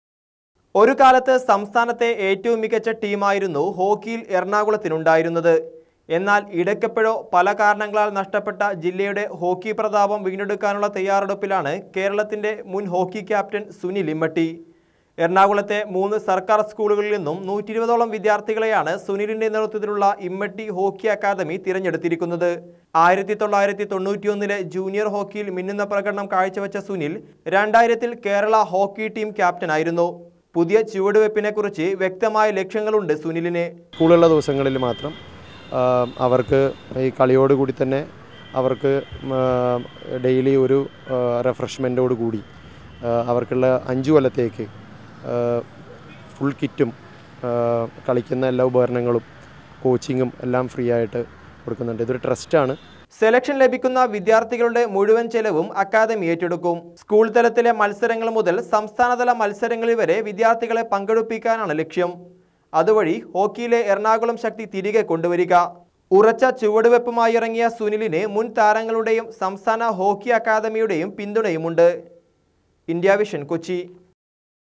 Hockey Academy by Sunil Immatty, gets high note
ഹോക്കിയില്‍ എറണാകുളത്തിന് നഷ്ടപ്പെട്ട പ്രതാപം വീണ്ടെടുക്കാനുള്ള തയ്യാറെടുപ്പിലാണ് കേരള ഹോക്കി ടീം മുന്‍ ക്യാപ്റ്റന്‍ സുനില്‍ ഇമ്മട്ടി. 120 സ്‌കൂള്‍ വിദ്യാര്‍ത്ഥികളെ തെരഞ്ഞെടുത്താണ് സുനിലിന്റെ നേതൃത്വത്തിലുള്ള ഇമ്മട്ടി ഹോക്കി അക്കാദമി പരിശീലനം നല്‍കുന്നത്.